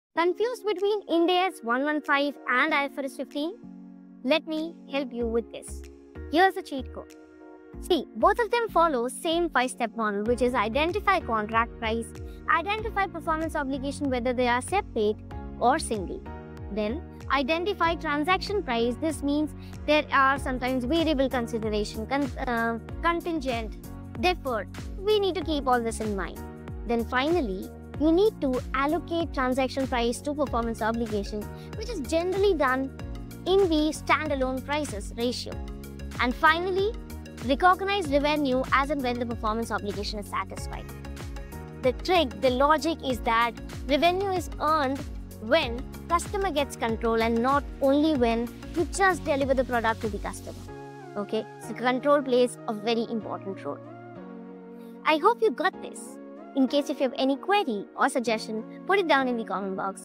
Ind AS 115 vs IFRS 15: Revenue Recognition Explained in 60 Seconds! || By CA Swati Gupta
Click on the link to register and learn IFRS 15 in detail Download application for more details